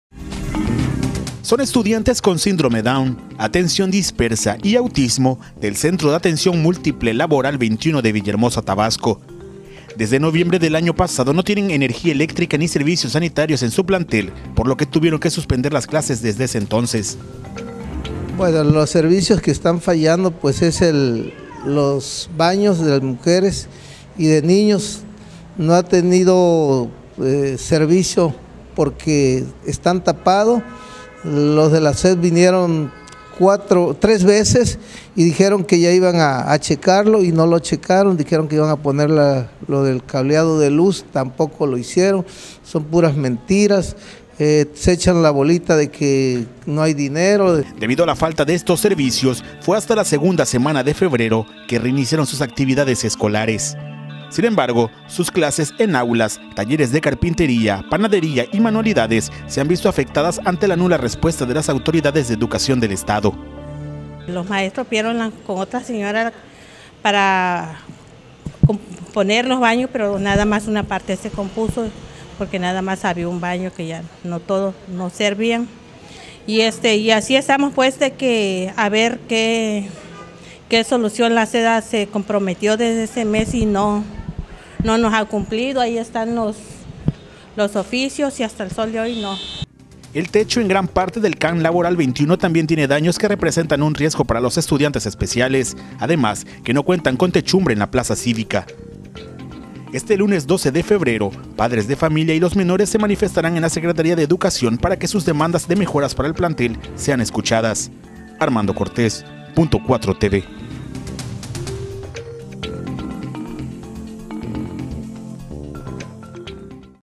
ESCUELA ESPECIAL DE TABASCO SIN SERVICIOS
La Secretaría de Educación de Tabasco ignora a niños con discapacidad del CAM Laboral 21 de Villahermosa, desde hace meses su escuela carece de servicios básicos.

Información: Armando Cortez
Imágenes: Jimmy Romero
Edición: Jesús de la Cruz